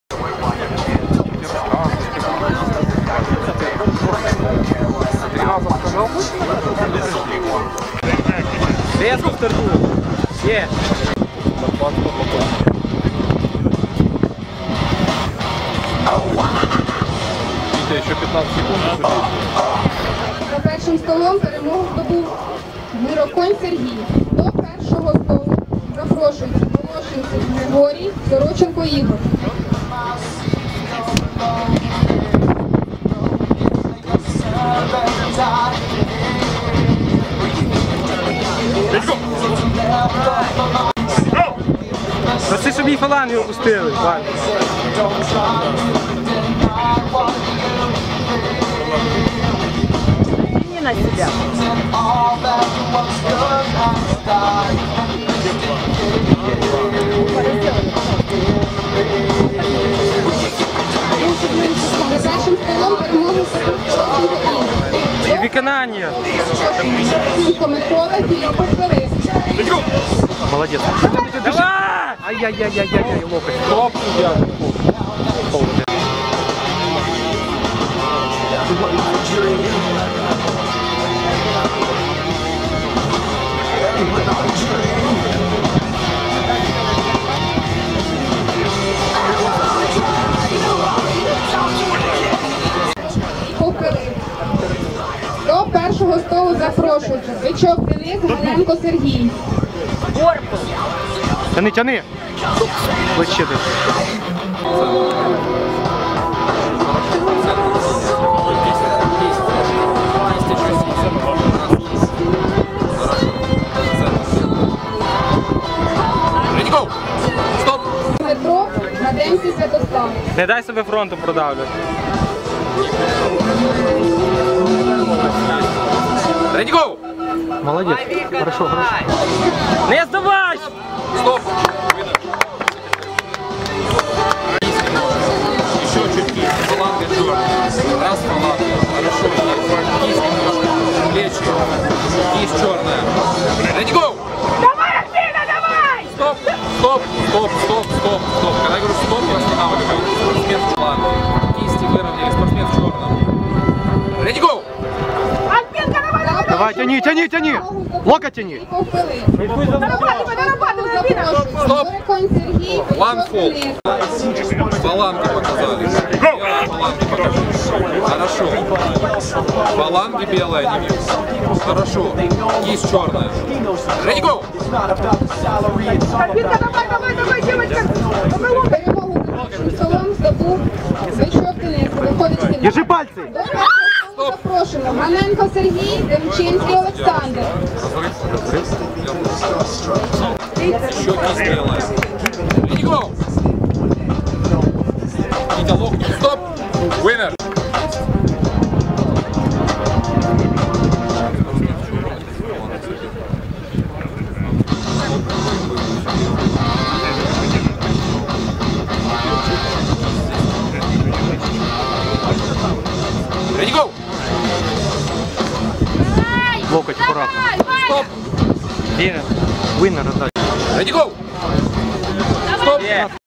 Кубок Київської області з армспорту - в.к.: Ж+60 (ліва)
Кубок Київської області з армспорту - вагова категорія: Жінки понад 60 кг. Ліва рука
Фастів, стадіон "Машинобудівник".